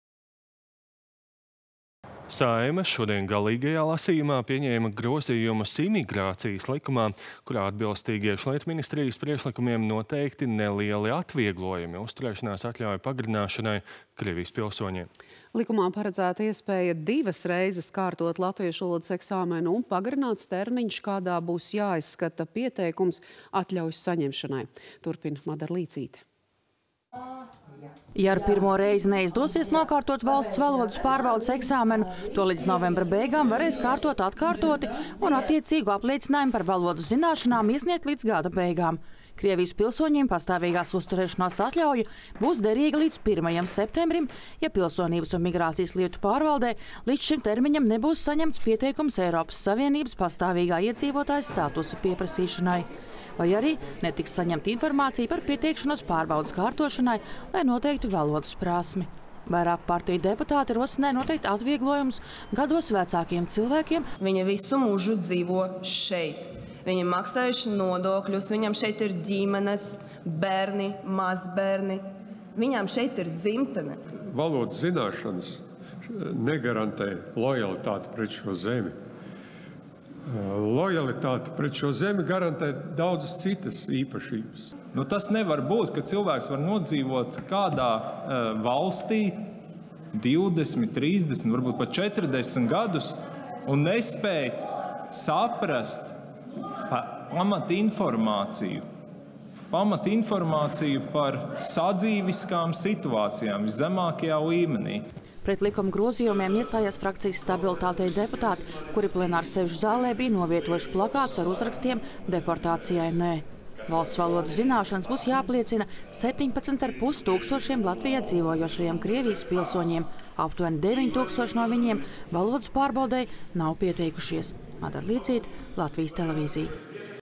Dod papildu laiku atkārtotai valodas pārbaudei
Saeima šodien galīgajā lasījumā pieņēma grozījumus Imigrācijas likumā, kurā atbilstīgi Iekšlietu ministrijas priekšlikumiem noteikti nelieli atvieglojumi uzturēšanās atļauju pagarināšanai Krievijas pilsoņiem. Likumā paredzēta iespēja divas reizes kārtot latviešu valodas eksāmenu, un pagarināts termiņš, kādā būs jāizskata pieteikums atļaujas saņemšanai. Turpina Madara Līcīte.